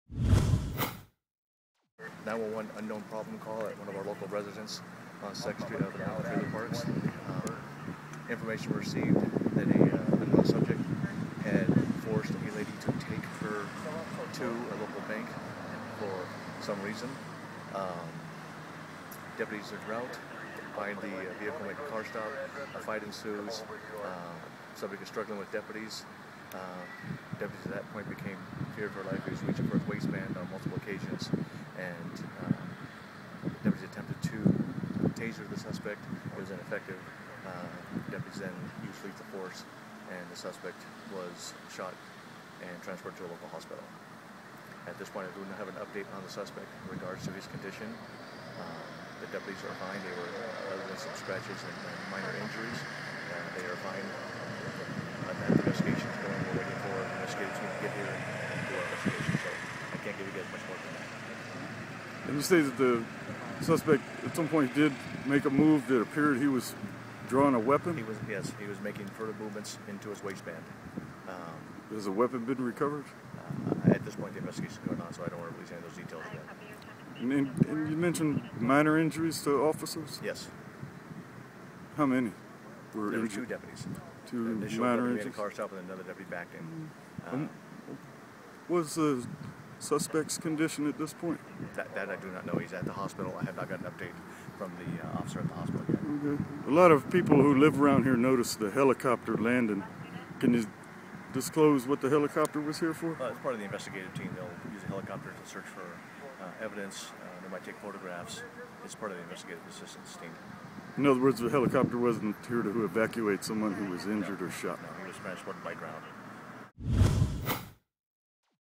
Yucaipa DIS
The San Bernardino County Sheriff's Department early Sunday identified a suspect fatally shot by deputies June 29 in a church parking lot and released more detail from law enforcement accounts of what preceded the shooting.

The man named by sheriff's officials, Merlin Factor, 25, was a resident of Yucaipa, according to residents and a woman who identified herself as Factor's mother.
 "That was my son," she wrote in a post on Redlands-Loma Linda Patch early Sunday. "He lived within a block of there with me as he was growing up at his Gramma's."

Redlands-Loma Linda Patch video by Guy McCarthy.